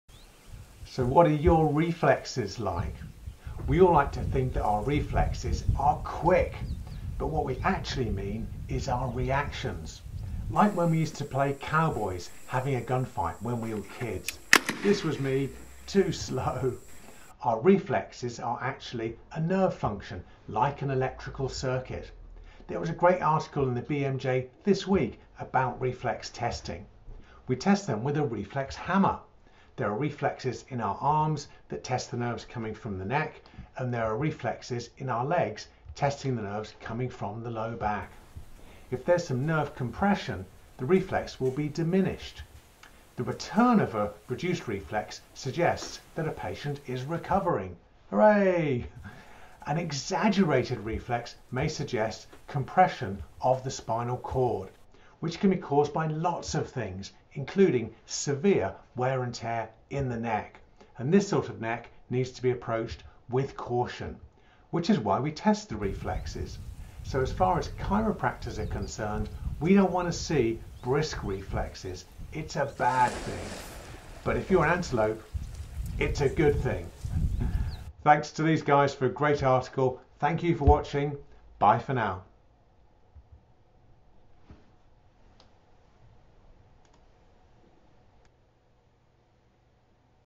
How quick are your reflexes?
A chat about the differences between reflexes and reactions. Is it a good idea to have brisk reflexes? Find out in this short video.